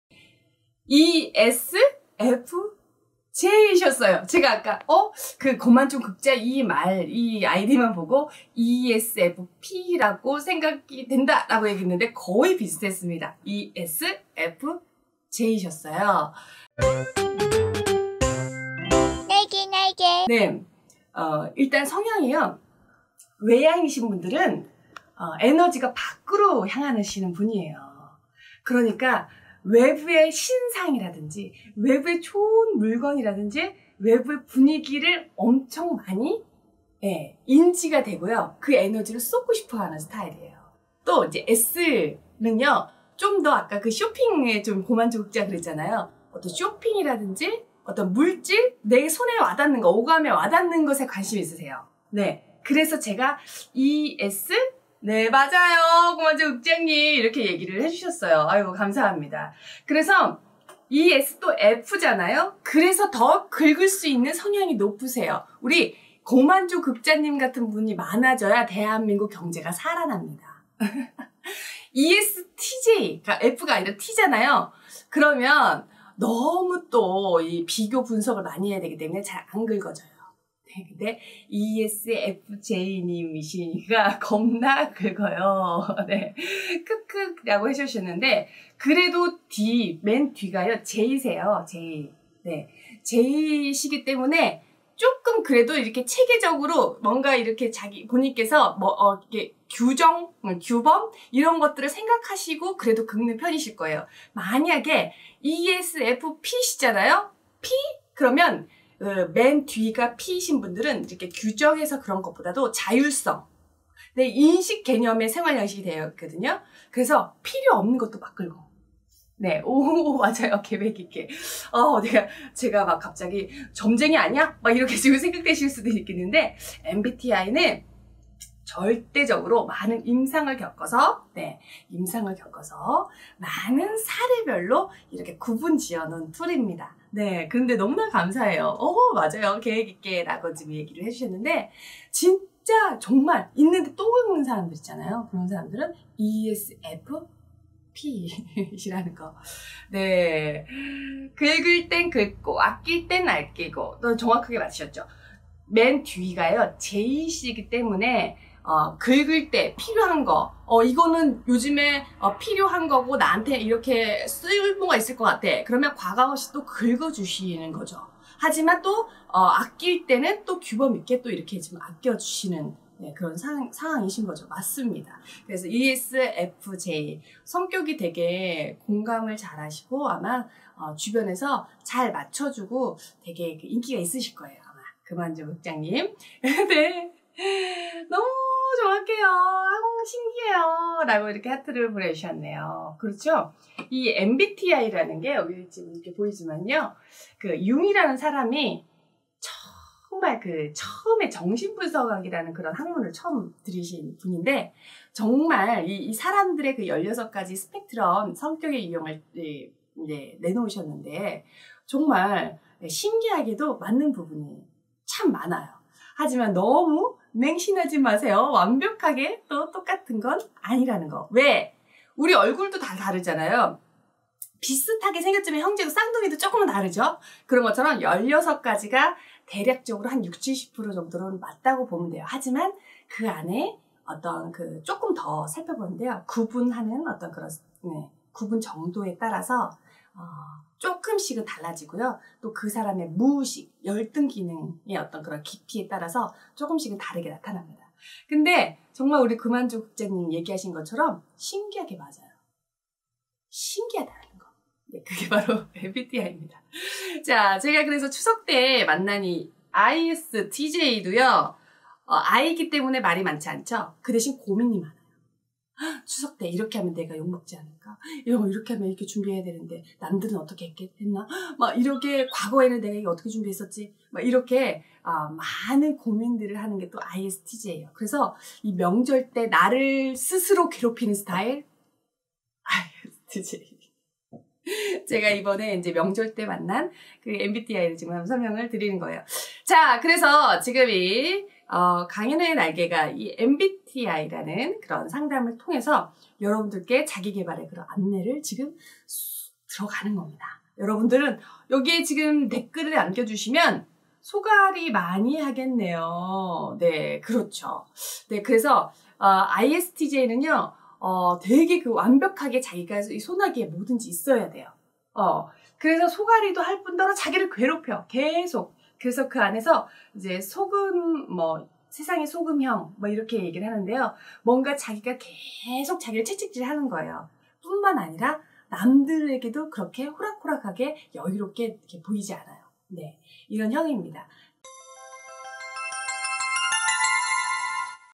물건을 보고 살까 말까하는 MBTI 성격유형은 바로! ESFJ
AI시대~나만의컨텐츠로 살아남기!!
심리테스트 강연 N잡러~
무료스터디 오픈방 입장클릭!!

라이브 커머스 방송중! MBTI 16가지 성격유형 중ESFJ 성격유형을 가지고 계신 고객님과의 상담입니다.

더 자세한 내용이 궁금하시면 아래클릭!!